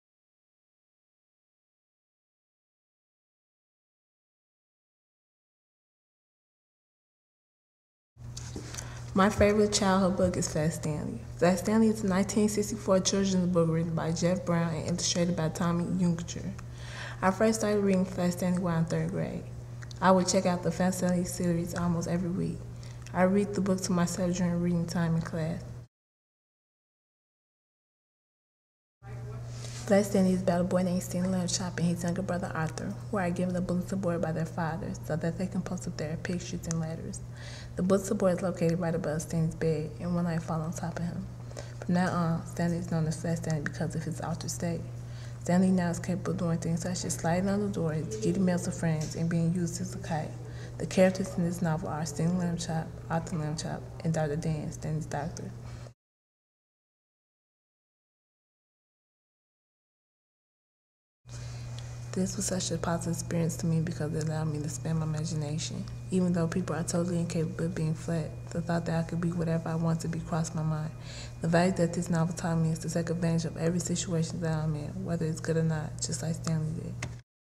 Flat Stanley - Favorite Childhood Book
Student presents a favorite book from her childhood in an independent video project.